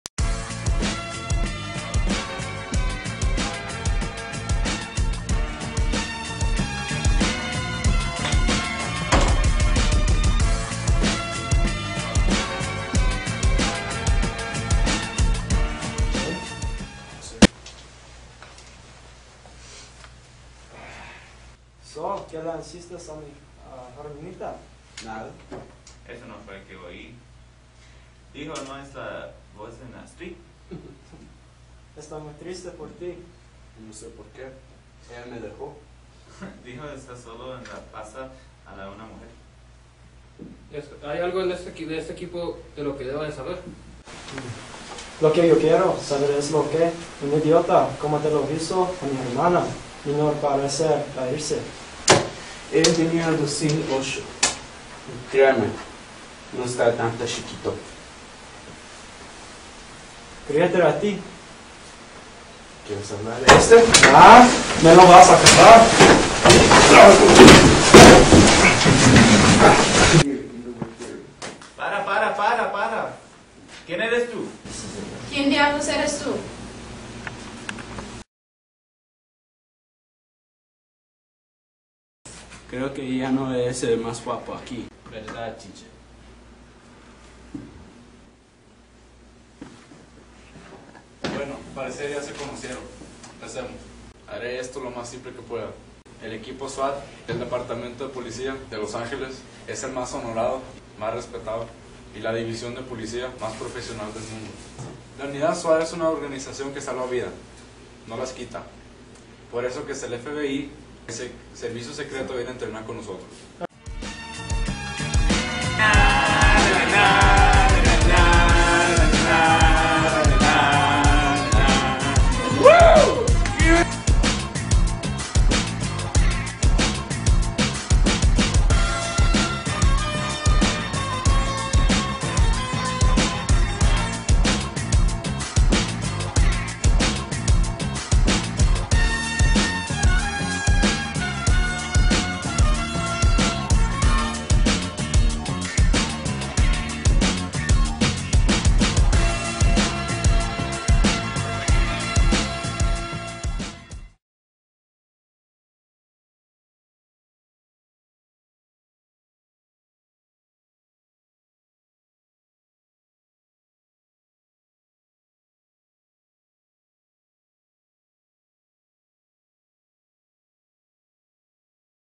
SWAT in Spanish, a remake by high school students
This was for an old school project. it when well, back in 2010...
Got an A